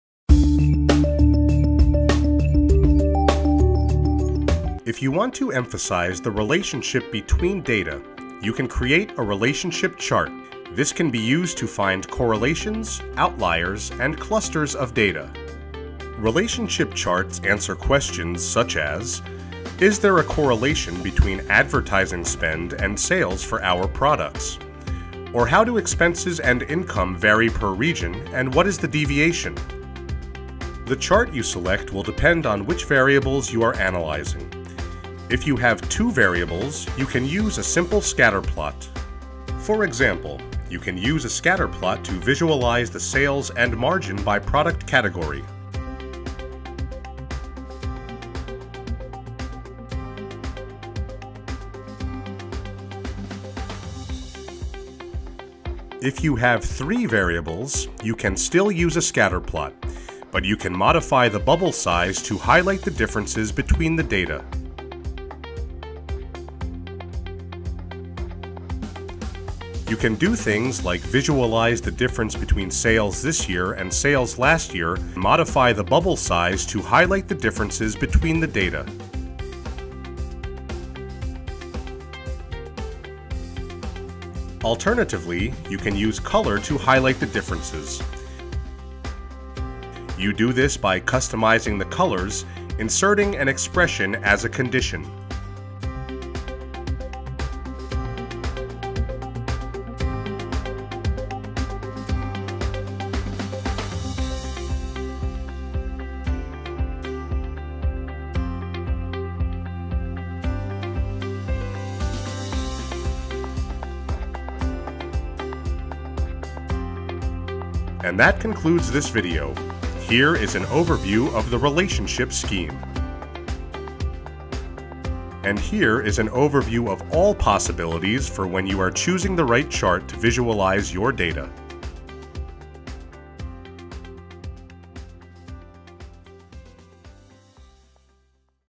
Choose the right chart - Relationship
If you want to emphasize the relationship between data, you can use a relationship chart. This can be used to find correlations, outliers and clusters of data.

Relationship charts answer questions such as: “is there a correlation between advertising spend and sales for our products?
Or, how do expenses and income vary per region and what’s the deviation?